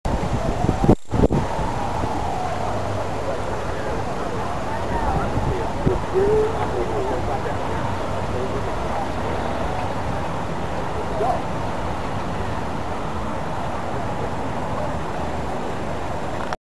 Collegeville Perkiomen Creek - Day After Irene - Perkiomen Hotel - 8/28/11
Intersection of Route 29 and Ridge Pike in Collegeville.